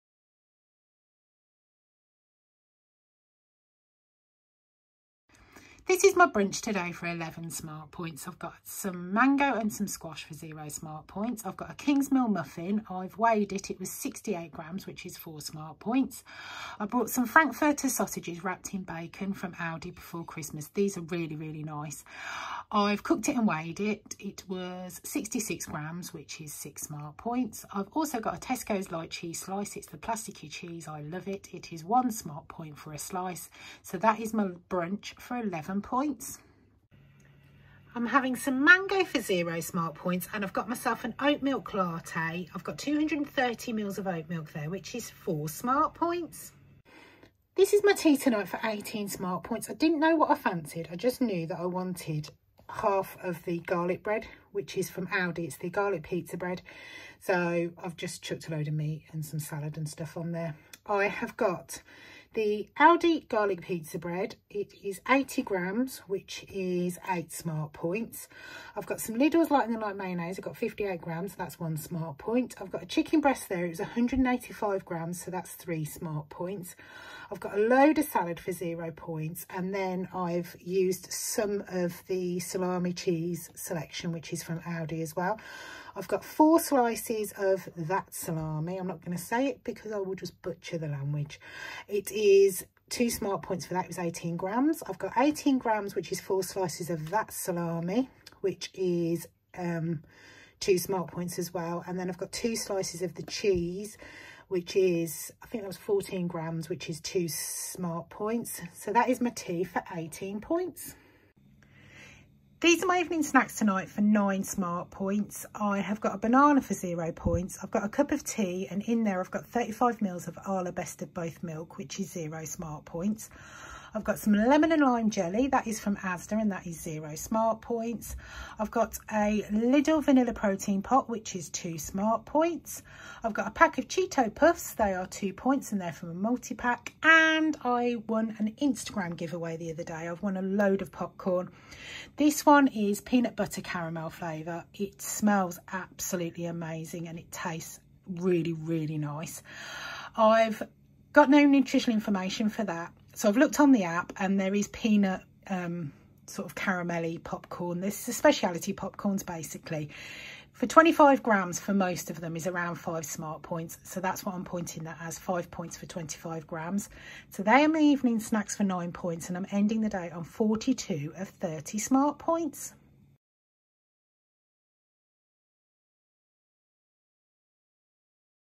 250lbs lost. What I eat in a day on WW (weight watchers) to lose weight
42/30sp weight watchers uk smartpoints used today.

What I eat in a day on weight watchers green plan. To lose weight

Really enjoyed my food today.

Lost 250lbs eating real food, including take always. With WW nothing is off limits.
Started on slimming world and switched to ww uk. I have been on my weight loss journey for over 5 years

Took three years to get to my goal weight. And I’ve been maintaining for 2 years